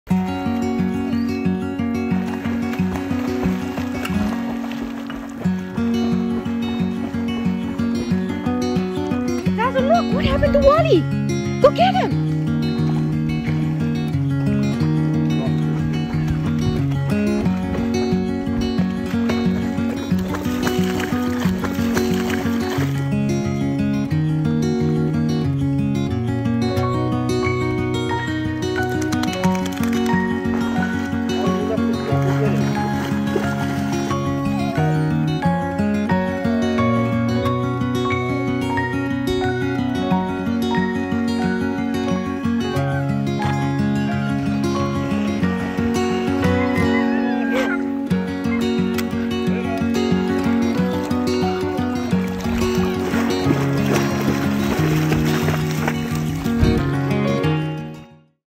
Labrador acting lifeguard to brother ❤️
Zazu our Labrador is a great swimming and Walle our German Shorthaired Pointer is still working on his swimming skills. We threw a stick and Walle went to go get it, but was a little too far for him so he couldn't get it. Zazu the big brother that he is always watching Walle saw what was going on and went to go get the stick for Walle. Walle then was prancing around with the stick he got.